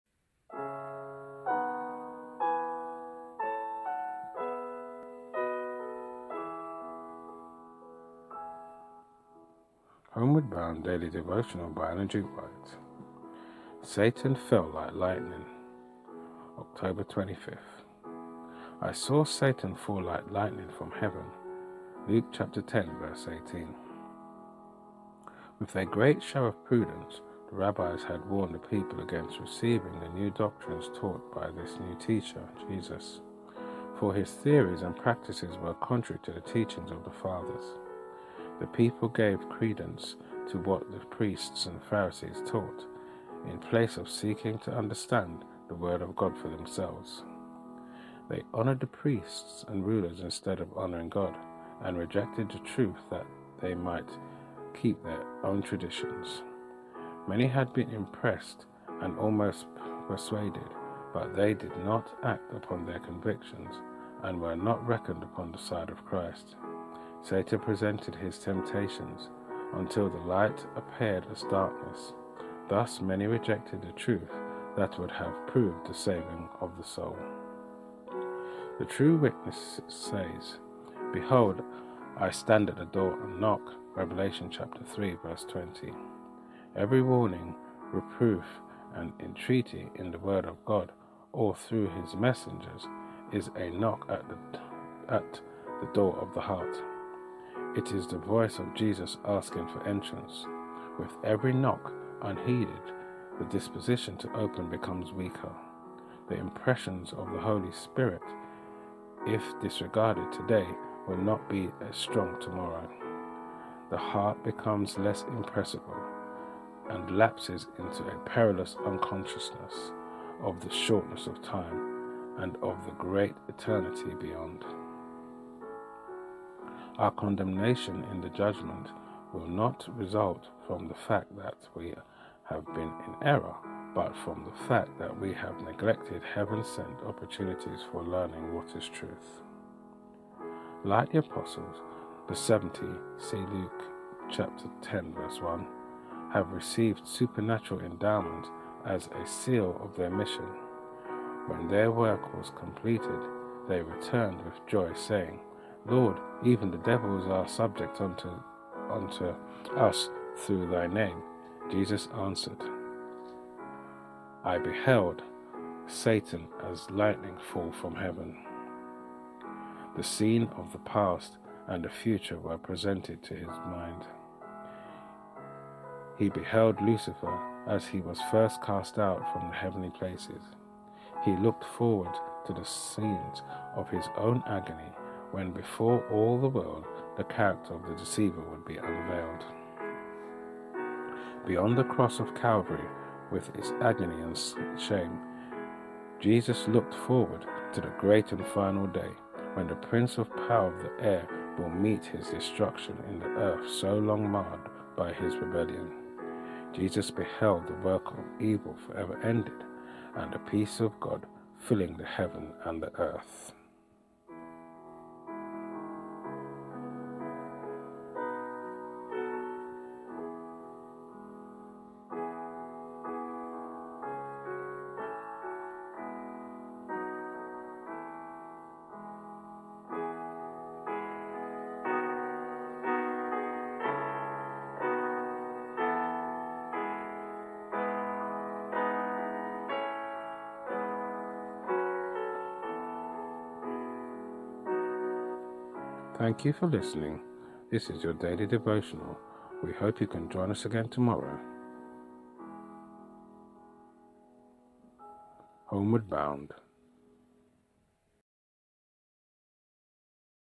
Homeward Bound: Satan Fell Like Lightning, October 25th
Daily Devotional Reading By Ellen G White. Homeward Bound: Satan Fell Like Lightning, October 25th

_I saw Satan fall like lightning from heaven._ Luke 10:18. HB 326.1

With a great show of prudence the rabbis had warned the people against receiving the new doctrines taught by this new teacher [Jesus]; for His theories and practices were contrary to the teachings of the fathers. *The people gave credence to what the priests and Pharisees taught, in place of seeking to understand the word of God for themselves. They honoured the priests and rulers instead of honouring God, and rejected the truth that they might keep their own traditions.* Many had been impressed and almost persuaded; but they did not act upon their convictions, and were not reckoned on the side of Christ. Satan presented his temptations, until the light appeared as darkness. Thus many rejected the truth that would have proved the saving of the soul. HB 326.2

The True Witness says, _“Behold, I stand at the door, and knock.”_ (Revelation 3:20.) *Every warning, reproof, and entreaty in the word of God or through His messengers is a knock at the door of the heart. It is the voice of Jesus asking for entrance. With every knock unheeded, the disposition to open becomes weaker. The impressions of the Holy Spirit if disregarded today, will not be as strong tomorrow.* The heart becomes less impressible, and lapses into a perilous unconsciousness of the shortness of life, and of the great eternity beyond. Our condemnation in the judgment will not result from the fact that we have been in error, but from the fact that we have neglected heaven-sent opportunities for learning what is truth. HB 326.3

Like the apostles, the seventy [see Luke 10:1] had received supernatural endowments as a seal of their mission. When their work was completed, they returned with joy, saying, _“Lord, even the devils are subject unto us through Thy name.”_ Jesus answered, _“I beheld Satan as lightning fall from heaven.”_ The scenes of the past and the future were presented to the mind of Jesus. He beheld Lucifer as he was first cast out from the heavenly places. He looked forward to the scenes of His own agony, when before all the worlds the character of the deceiver should be unveiled. . . . HB 326.4

Beyond the cross of Calvary, with its agony and shame, Jesus looked forward to the great final day, when the prince of the power of the air will meet his destruction in the earth so long marred by his rebellion. *Jesus beheld the work of evil forever ended, and the peace of God filling heaven and earth.* —The Desire of Ages, 489, 490. HB 326.5

Further Study Pinned in comments